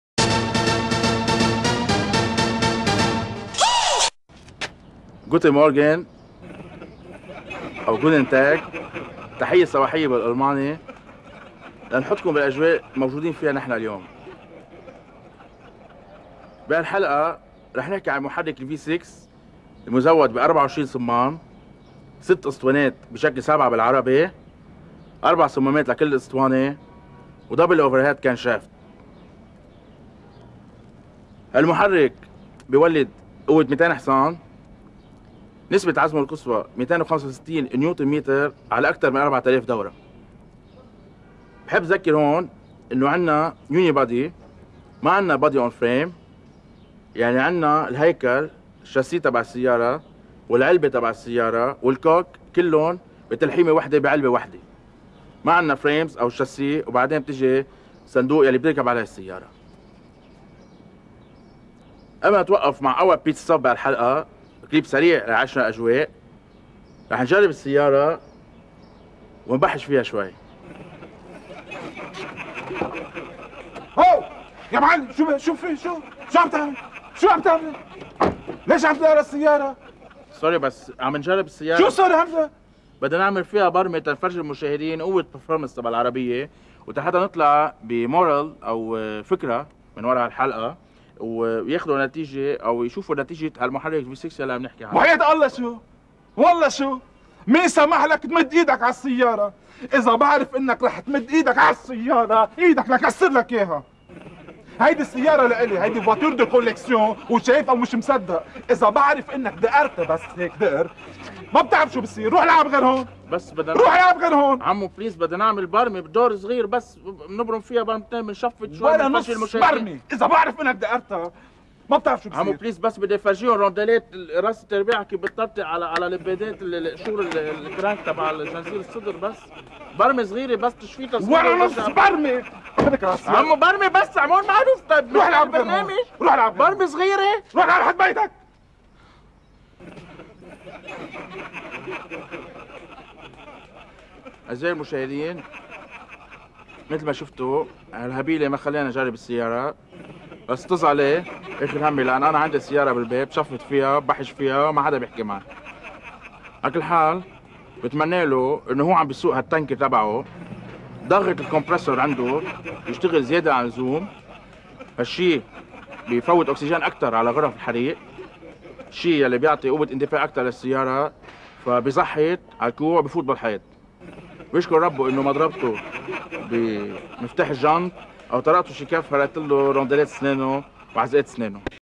SLCHI - Fadi Raidy Motor Show Parody (Nadim Mhanna)
Fadi Raidy Mocking Nadim Mhanna - Motor SHow MTV